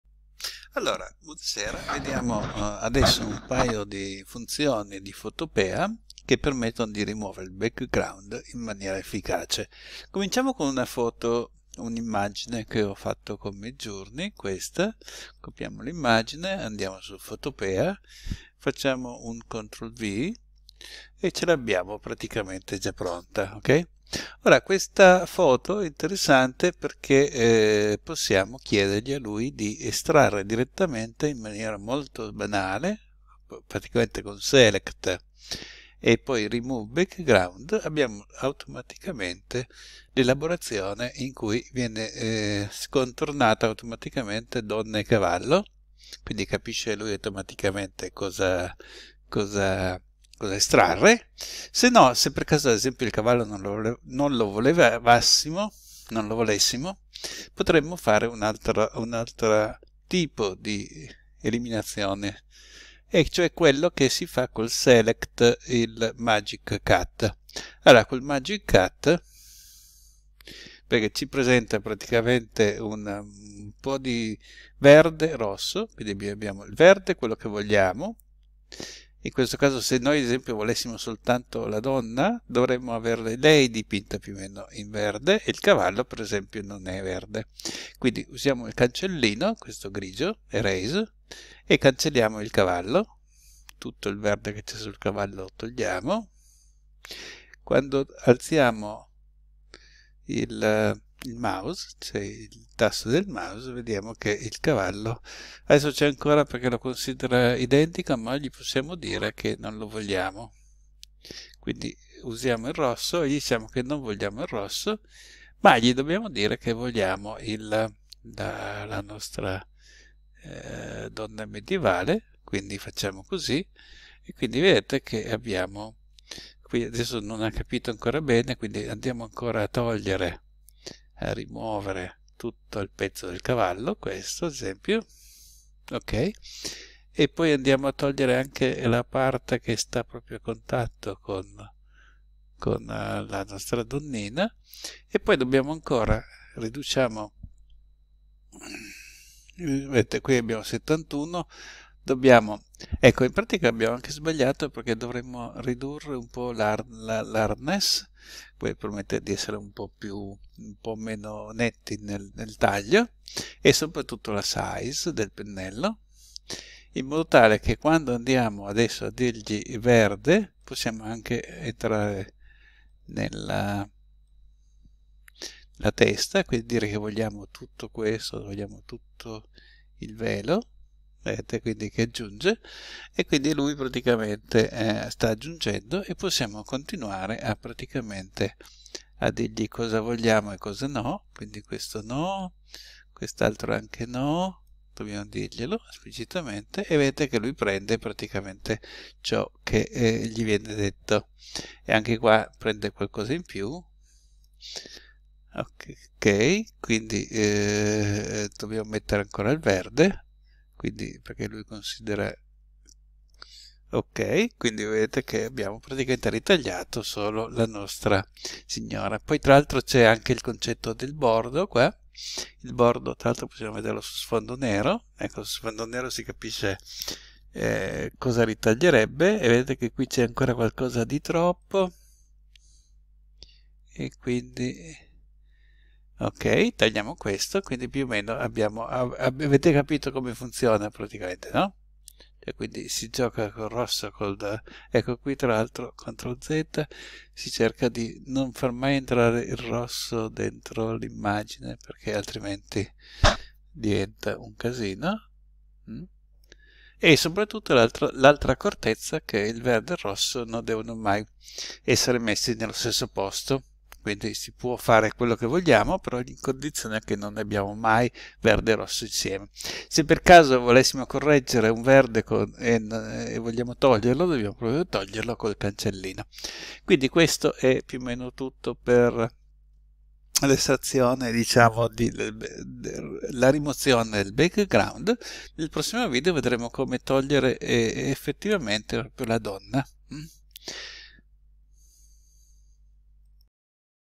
Photopea - rimuovi sfondo
📚 **Guida alla Rimozione del Background con Photopea**

### Capitolo 1: Rimozione Automatica del Background
1. **Preparazione dell'Immagine**
   - Seleziona una foto (esempio: foto con donna e cavallo).
   - Copia l'immagine.
   - Apri Photopea e incolla l'immagine (Ctrl + V).

2. **Rimozione Automatica**
   - Usa la funzione "Select" seguita da "Remove Background".
   - Questo strumento rimuove il background intorno a donna e cavallo in modo automatico.

### Capitolo 2: Uso del Magic Cut per Rimozione Selettiva
1. **Selezione con Magic Cut**
   - Attiva lo strumento "Magic Cut".
   - Il Magic Cut mostra aree verdi (da mantenere) e rosse (da eliminare).

2. **Personalizzazione della Selezione**
   - Per mantenere solo la donna, colora in verde la donna e usa l'opzione "Erase" (cancellino grigio) per rimuovere il verde dal cavallo.
   - Al rilascio del mouse, se il cavallo è ancora presente, usa il colore rosso per marcarlo come elemento da eliminare.
   - Aggiungi rosso alle parti del cavallo e del background che vuoi escludere.

3. **Ajustamento della Selezione**
   - Modifica la "hardness" (durezza) per un taglio meno netto.
   - Ajusta la "Size" (dimensione) del pennello per maggiore precisione.
   - Continua ad aggiungere verde alle parti della donna che vuoi mantenere, incluso il velo.
   - Assicurati di marcare esplicitamente tutte le parti da escludere.

4. **Controllo del Bordo**
   - Verifica il risultato su uno sfondo nero per identificare eventuali errori.
   - Usa Ctrl + Z per annullare eventuali errori, soprattutto se il rosso invade l'immagine.
   - Ricorda: non sovrapporre verde e rosso nella stessa area. Se necessario, usa il cancellino per correggere.

5. **Risultato Finale**
   - Dopo aver seguito questi passaggi, otterrai un'immagine con solo la donna, senza cavallo né background.

🔜 Nel prossimo video, vedremo come rimuovere specificatamente la donna dall'immagine.